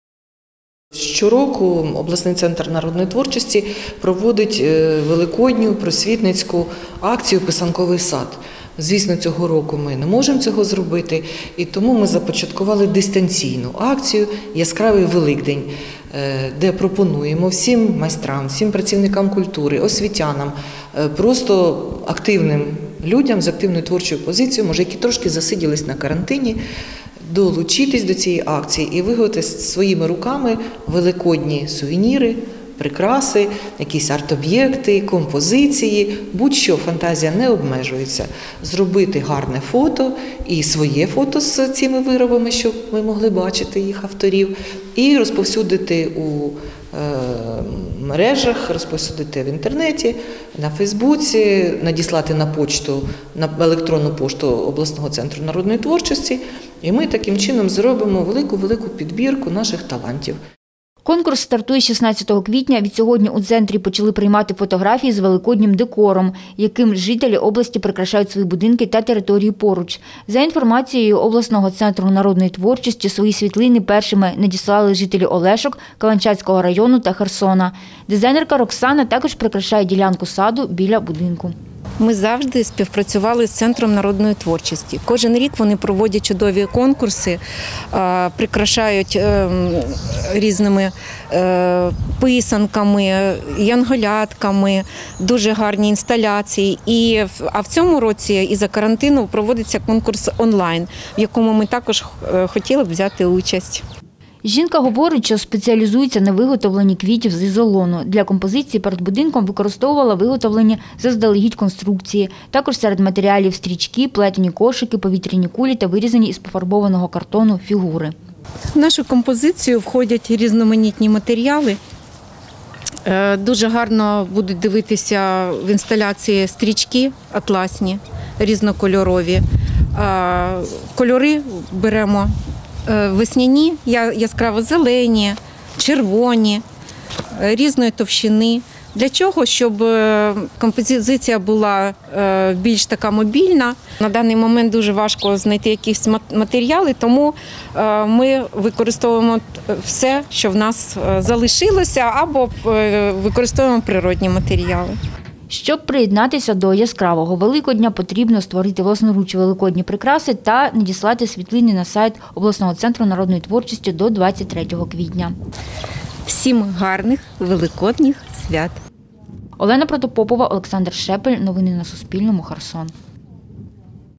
Херсонцям пропонують долучитися до Великоднього конкурсу
Обласний центр народної творчості  пропонує херсонцям та жителям області долучитися до Великоднього конкурсу. Про це кореспондентам Суспільного розповіла начальниця інформаційного відділу центру Наталія Вигоднер.